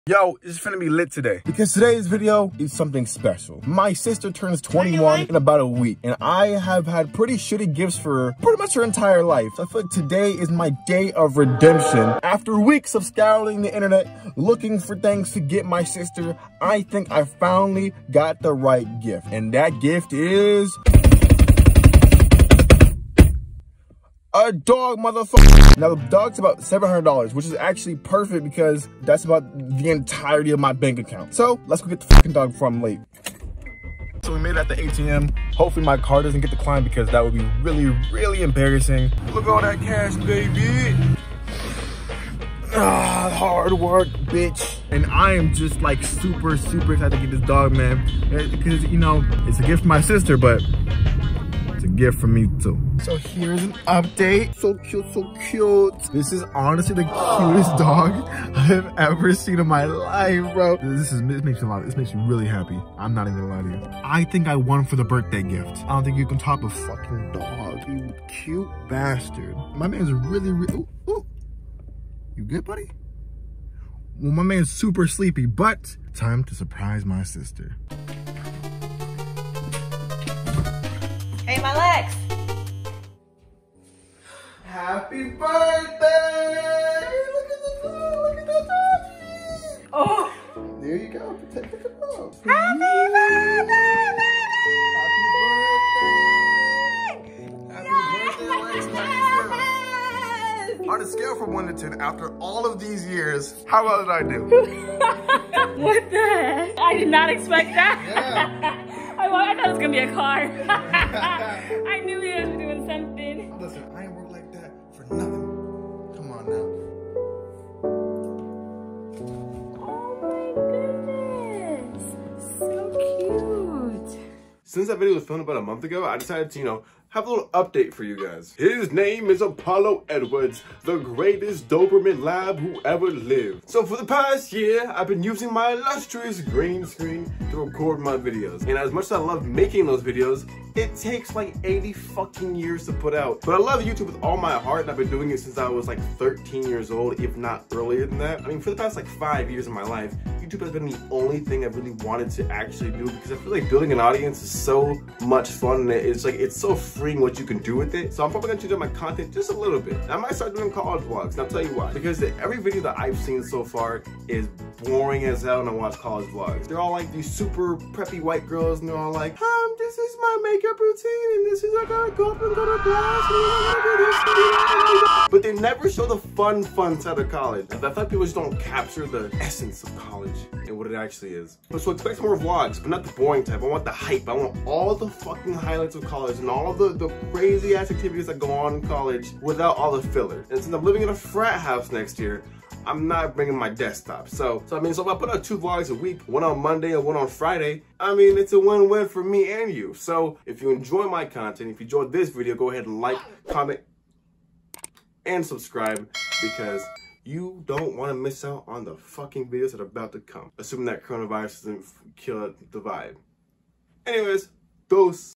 She Couldn’t Believe This!
I Adopted a puppy and gave it to my sister!!!

I'm 19 years into life & I have a real dream, vision, & plan. Watch this.